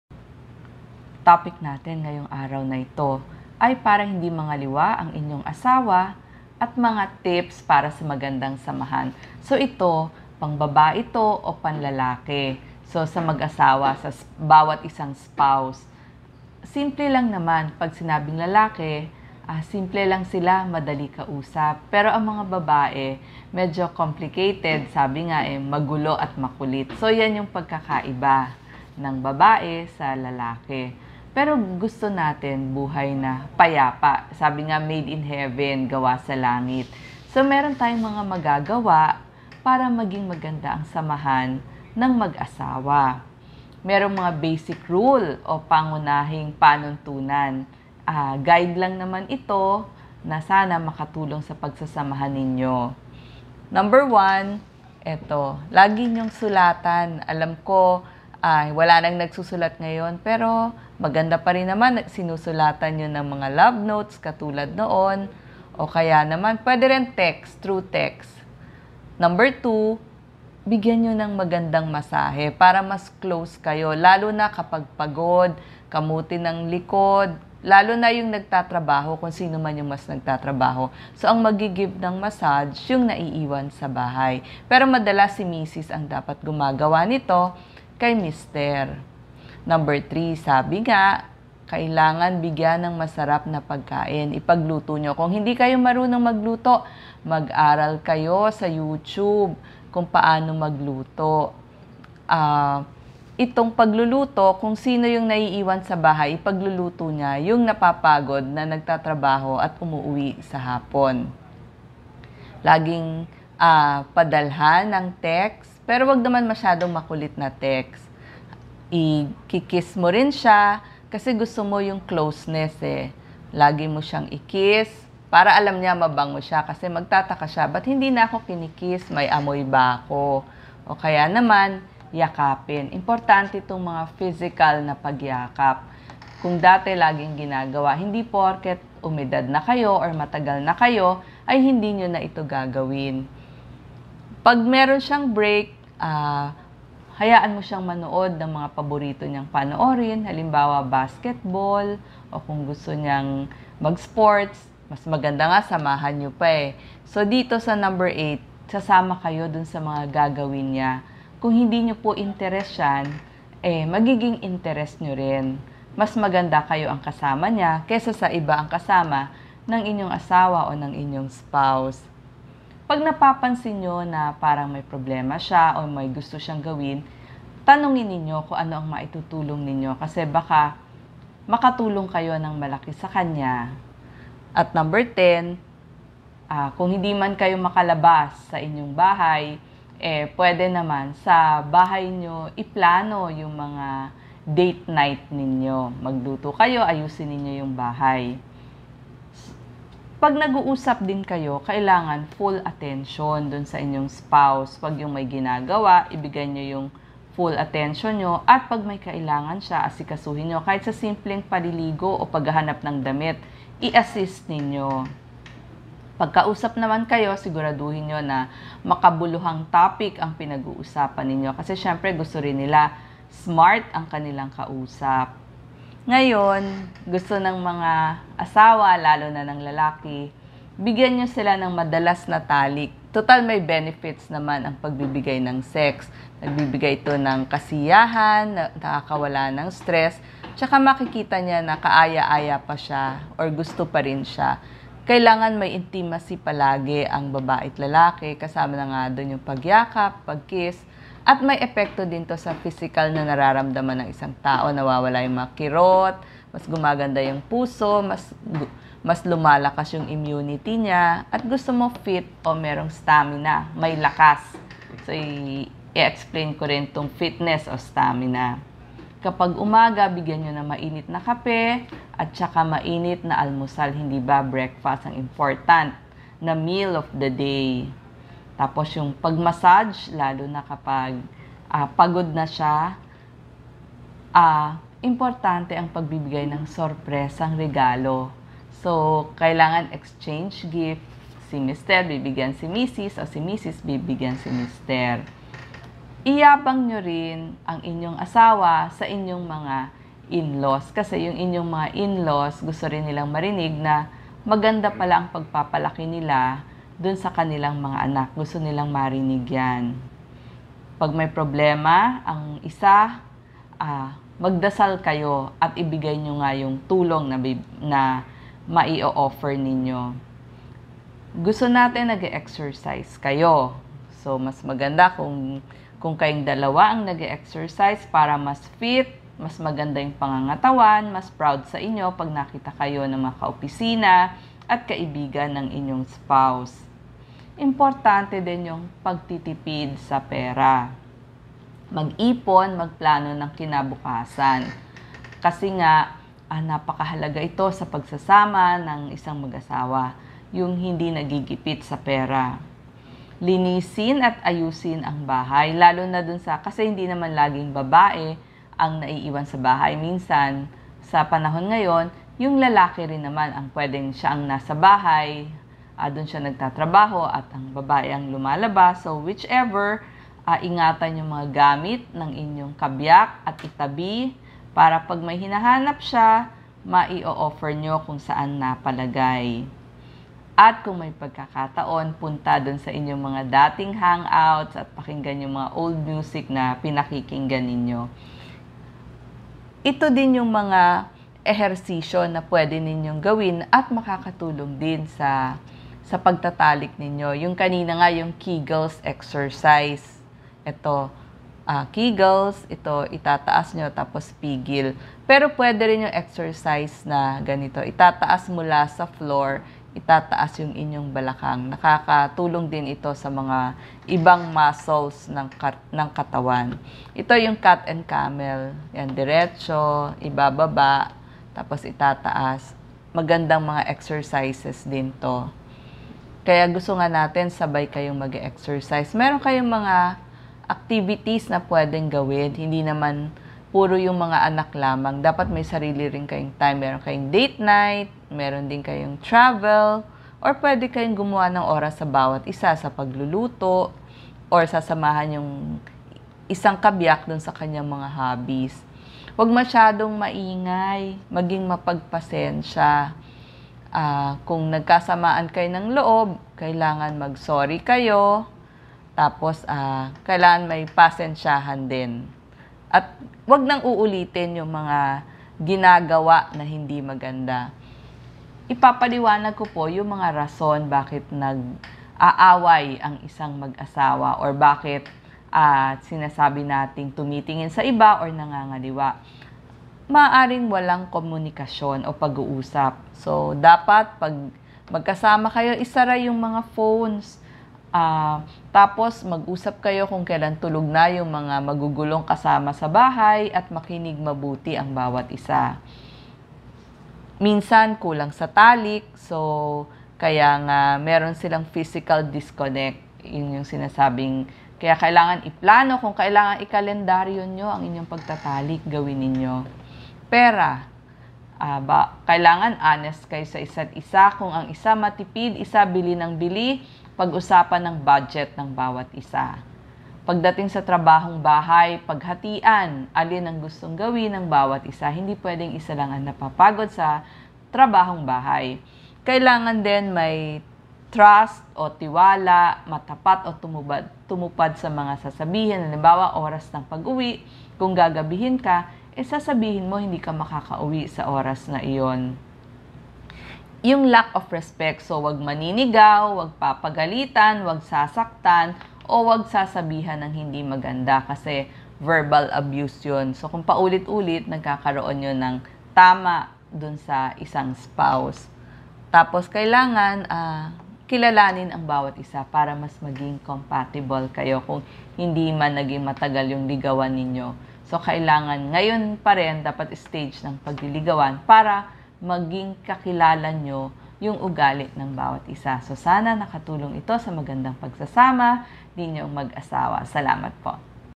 Bakit Nangaliwa ang Asawa - Payo ni Doc Liza Ramoso-Ong
Bakit Nangaliwa ang Asawa at Tips sa Maayos na Pagsasama
By Doc Liza Ong

1. Para gumanda ang samahan ng mag-asawa, mag-ehersisyo, i-masahe, panatilihin ang closeness at ituloy ang pagtatalik ng mag-asawa.
2. Ipag-luto at bigyan ng atensyon ang asawa.

Video